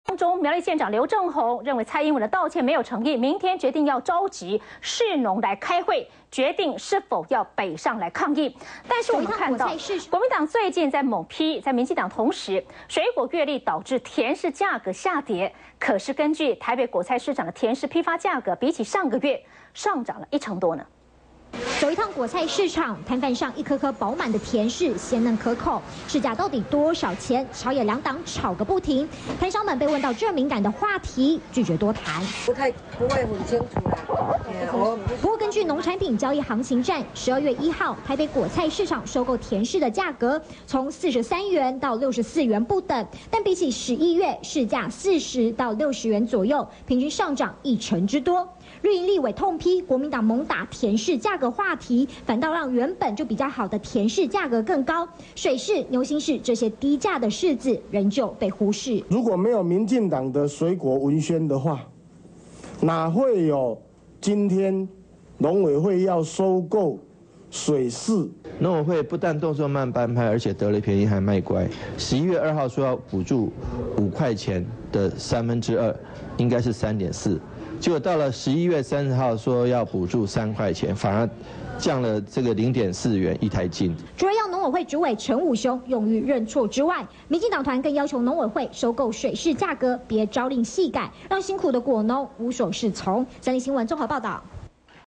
20111201 藍營猛打 柿價 高價甜柿漲近一成 三立新聞
請馬政府為農民生計著想　不要只有選舉的考量【2011/12/1】
在一開始，我要先講一個大家耳熟能詳的索羅門王故事。曾經有兩個母親為了一個孩子到底是誰的，在法官面前爭執不休，然而就在法官做出把孩子一分為二的提議之後，孩子的生母­­選擇鬆手，因為那是自己的心頭肉，沒有一位母親捨得讓自己的孩子受到任何的傷害。

這就是我現在的心情。

長久以來民進黨和農民站在一起，從照顧老農民的「老農津貼」開始，到執政後建立「九五機制」，讓我們的農民在生產過剩、產銷失衡的時候，有政府的補貼相挺，不必再獨自面對­­血本無歸的困境。我們從來沒有停止為農民朋友生計著想過，正因為民進黨來自這塊土地，如同母親看待自己的子女那樣看待辛勤耕作的每一位農民，不希望他們受到任何的傷害。­老­農津貼提高一千元的補助，也是一個例子。

我們清楚產銷失衡的問題才是這陣子水果價格風波的關鍵，而水柿確實也存在一台斤兩元的情況，嘉義縣水柿嚴重滯銷，也是事實，馬政府絕對有不可推卸的責任。但是為了不讓問題­模糊焦點，讓我們的對手有炒作的空間，對於文宣相片的誤植，我願意道歉。我也將要求競選團隊未來要更加謹慎。然而，站在一個母親的立場，連日來對手在選舉動機下的誇大炒作­，即使沒有對農產品的價格造成立即性的影響，但如果持續下去會讓外界模糊焦點，而且會讓政府應該扮演的角色也不清楚，反而對農民造成傷害。

馬政府持續而扭曲的政治操作，已經傷及無辜的農民，我們將持續關注農產品的產銷問題，為農民的權益把關。我也請民進黨執政縣市首長，在中央未挹注資源的情況下，仍然要盡最­大的努力來支持農民，支持水果的價格。

最後，我也要向所有的農民朋友保證，明年我當選總統，我不會再發生中央政府把照顧農民的責任推卸給地方的情況，新的政府團隊將會扛起所有的責任，以母親的心情照顧子女，為­農民的權益和幸福盡心盡力，這就是我的立場。至於，馬政府未來會不會放著產銷問題不解決，繼續操弄這些議題，我相信人民的眼睛是雪亮的，我們只能呼籲他們多為農民長久的生­計著想，不要只有一時選舉的考量。